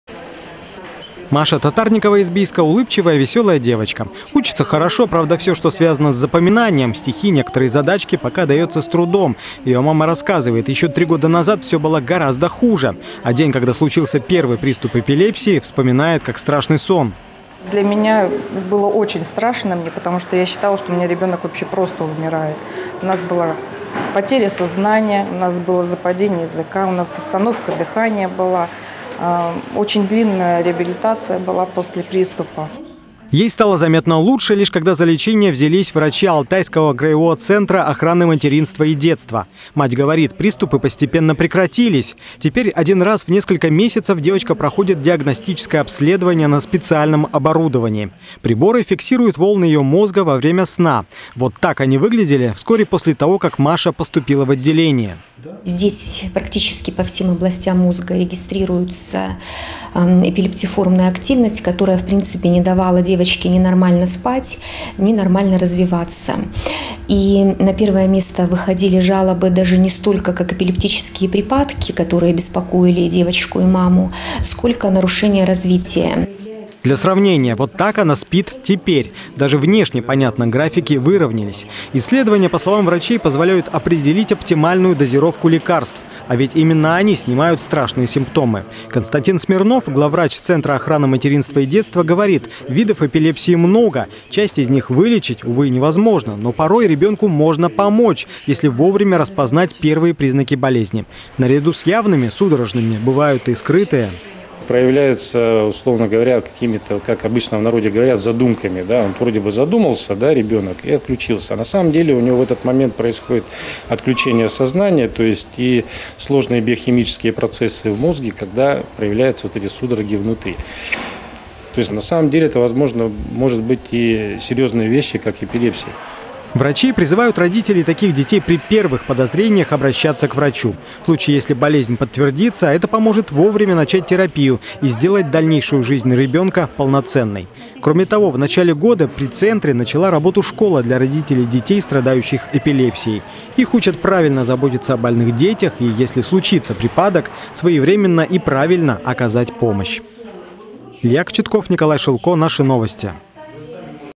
Алтайские медики рассказали о последних достижениях в лечении эпилепсии
Сегодня во всем мире отмечают… «фиолетовый день». Это ставшее с недавних пор традиционным название Дня  больных эпилепсией. В Алтайском крае от этого страшного недуга страдает около одного процента всего населения – больше 24 тысяч. Около 3 тысяч из них составляют дети.